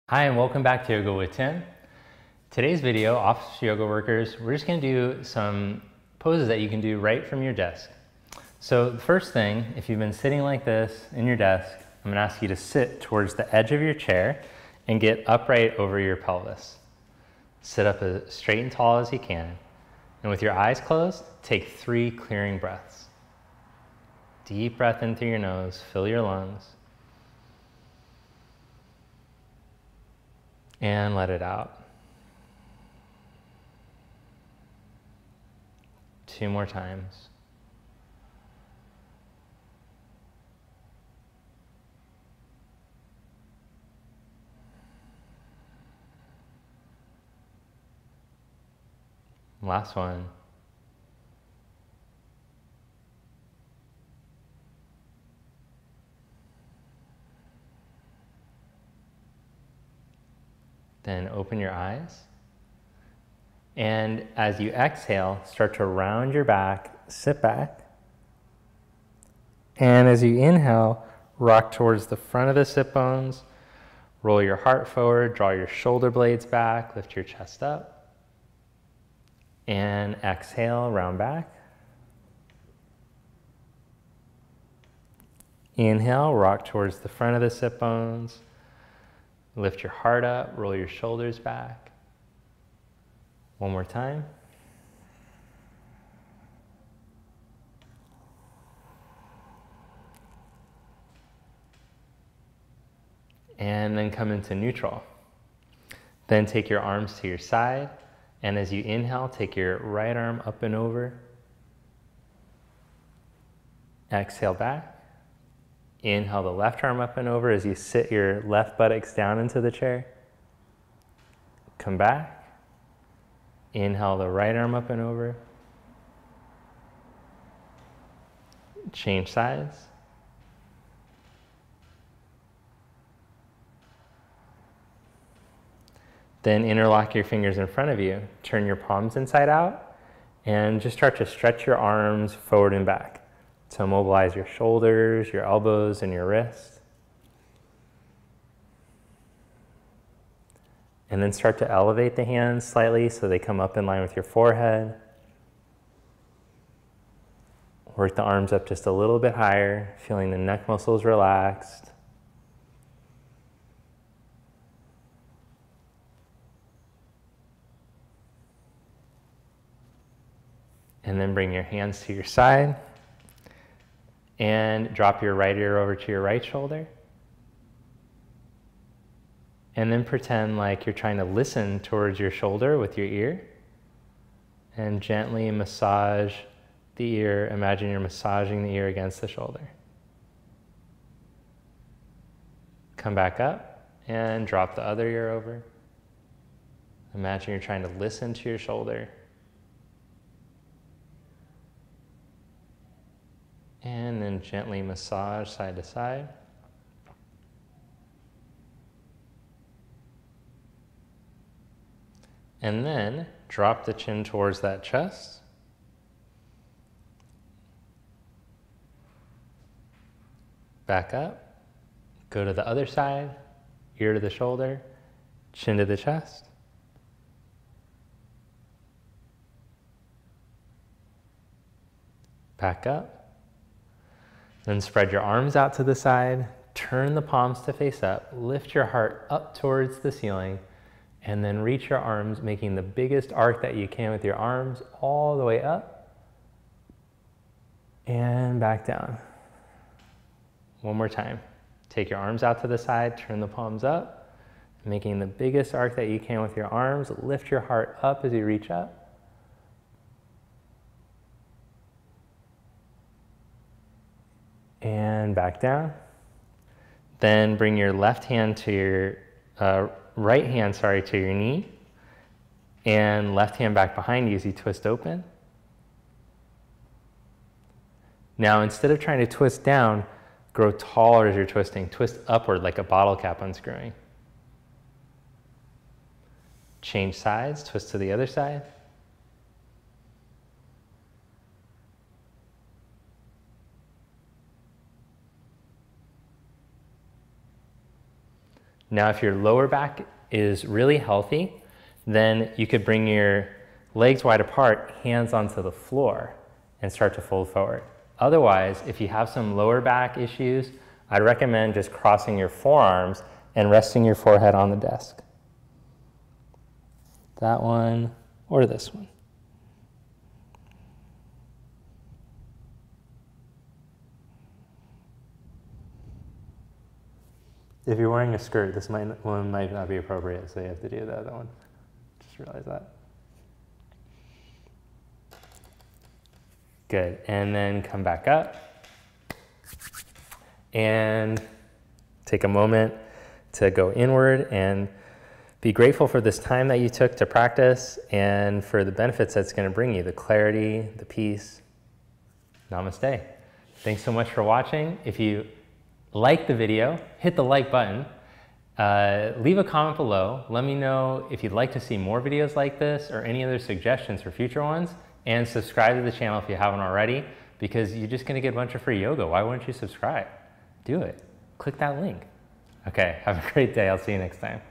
Yoga at Your Desk | Yoga With Tim
Yoga for Office Workers is a great Yoga at Your Desk sequence.

to sign up to become a member of the community.